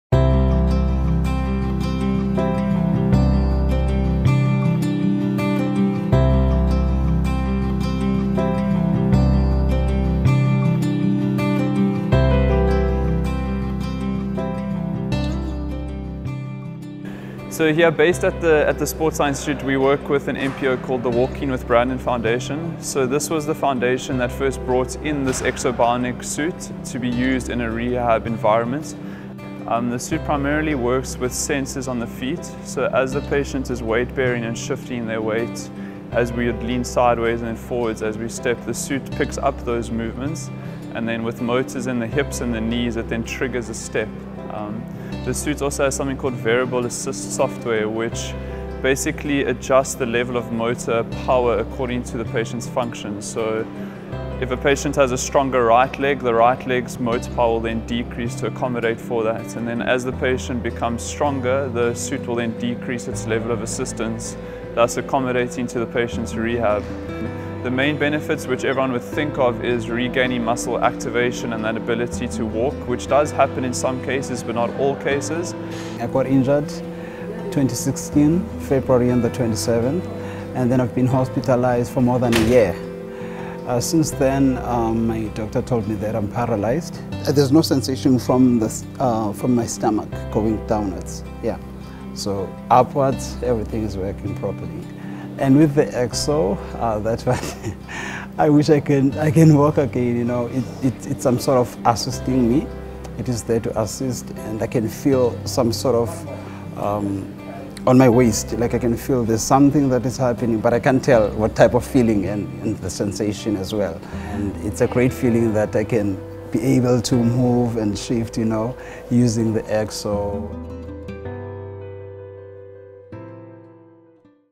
Ekso suit gives disabled patients new hope
Robert Evans, a PhD candidate in exercise science and sports medicine at UCT, explains the benefits of the ekso suit, a mobility device used in physical rehabilitation.

The suit, controlled by a trained operator, is used in rehabilitation programmes around the world. It stabilises patients' joints and helps strengthen weakened muscles.